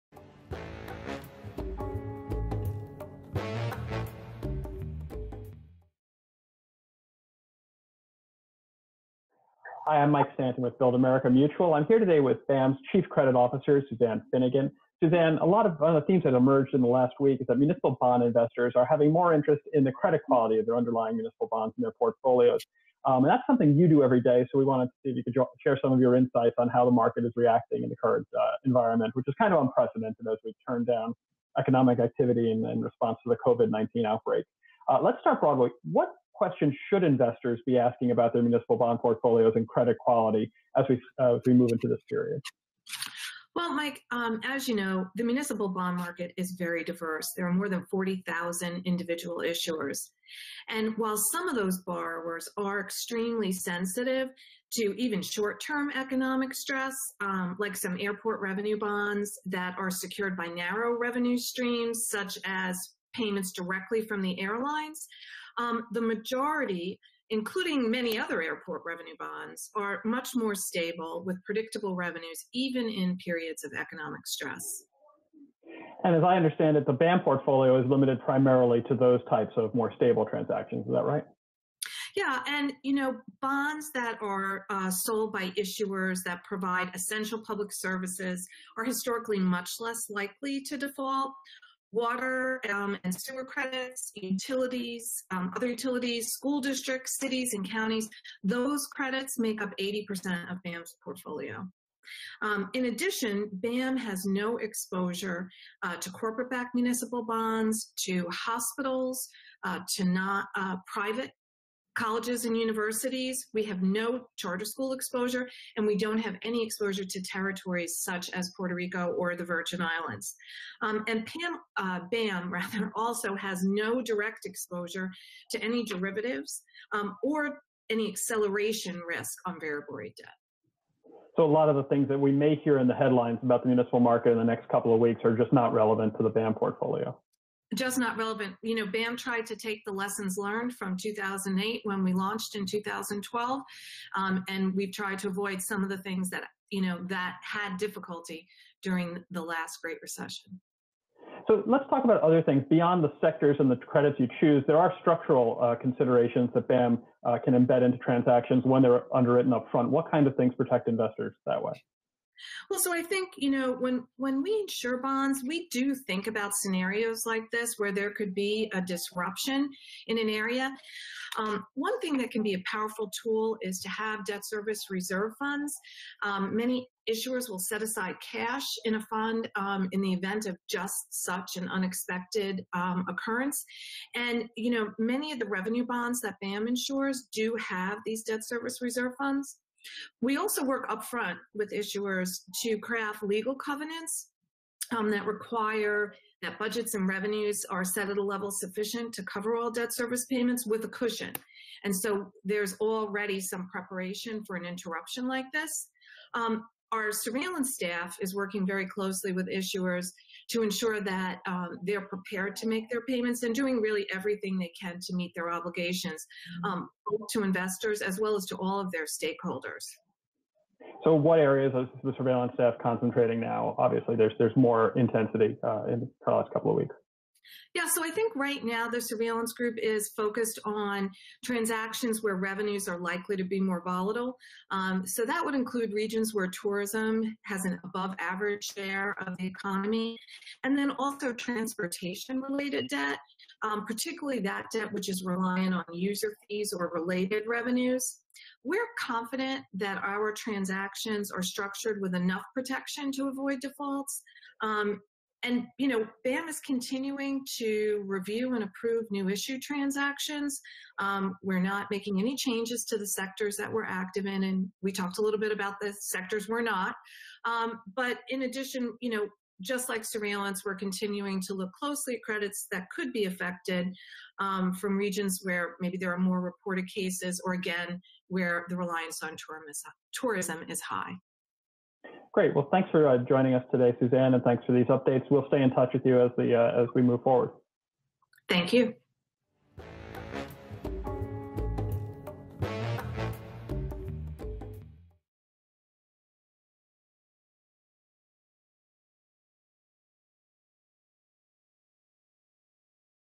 Municipal Bonds and COVID-19: Frequently Asked Questions
States and local governments across the U.S. are taking aggressive steps to limit spread of the novel coronavirus and curb cases of COVID-19. Build America Mutual Chief Credit Officer Suzanne Finnegan discusses the potential implications of these steps for municipal bond credit quality, and explains how BAM's $62 billion insured portfolio is protected from these risks.